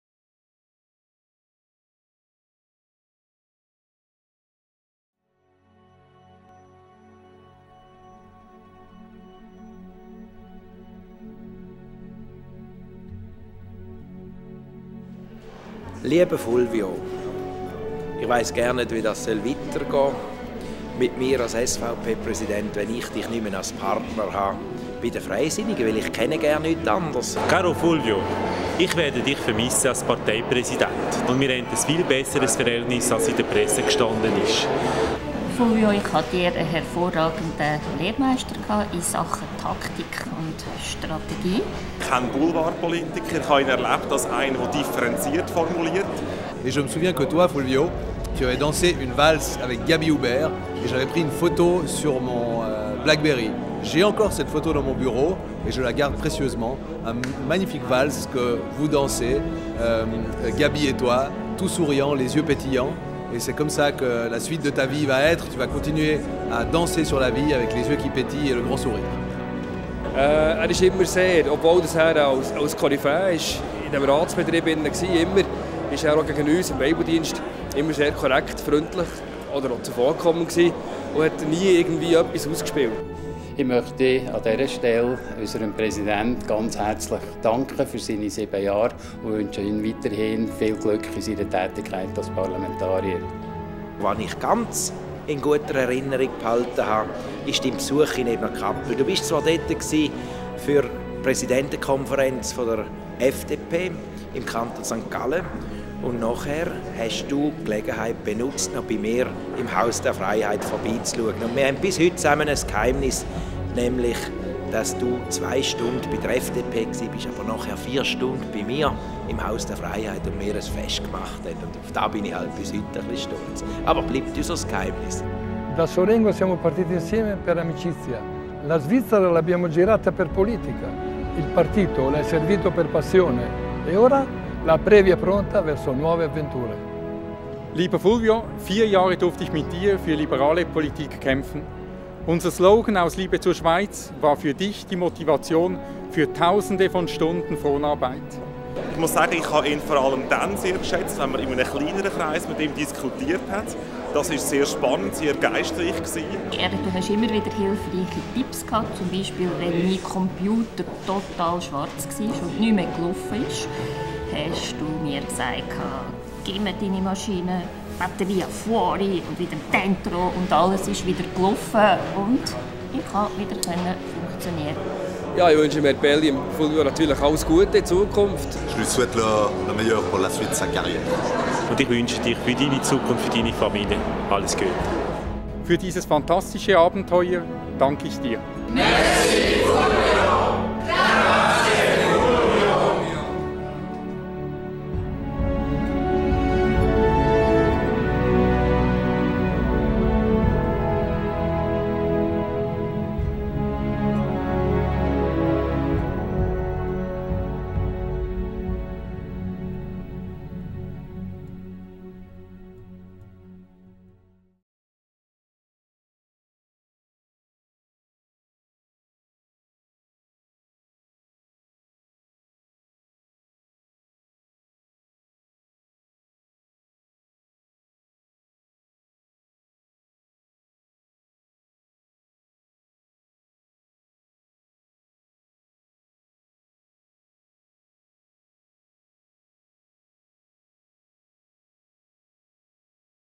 FDPtv: Grazie Fulvio
Verabschiedungsfilm für den abgetretenen FDP-Parteipräsident Fulvio Pelli